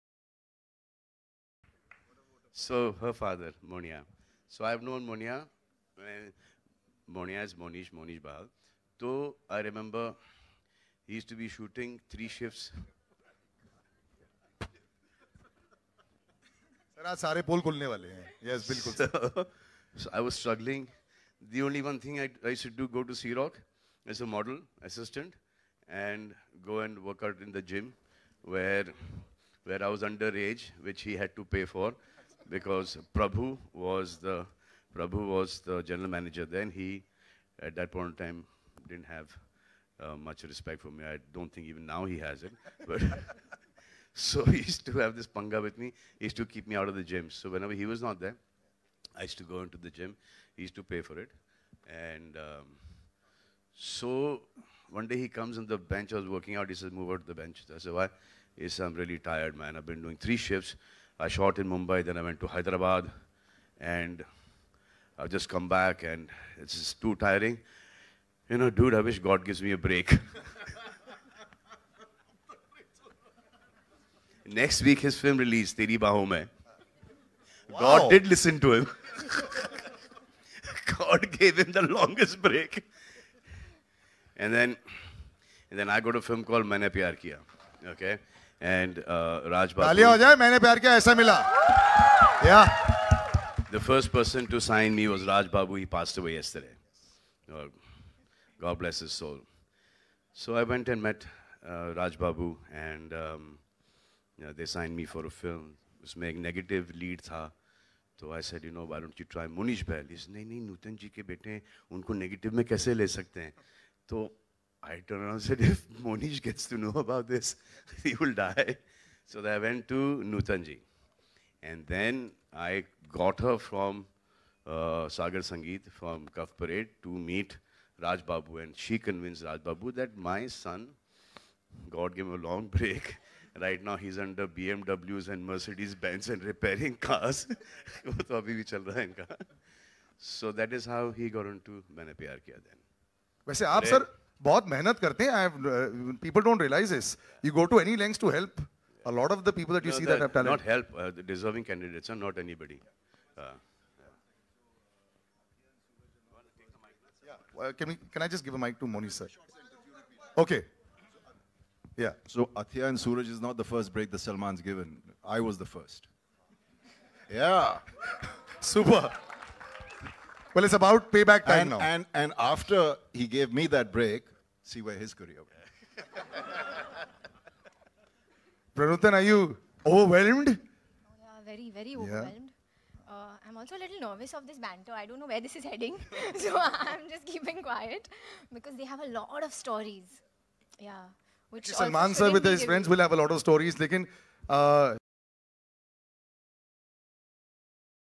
Salman Khan Makes Fun Of Mohnish Bahl In front of Daughter Pranutan and Media
Watch SalmanKhan making fun of his Co-star and friend MohnishBahl in front of his daughter Pranutan Bahl who will be seen debuting in Salman Khan home production film Notebook.

Abhishek Halder
Edited by Abhishek Halder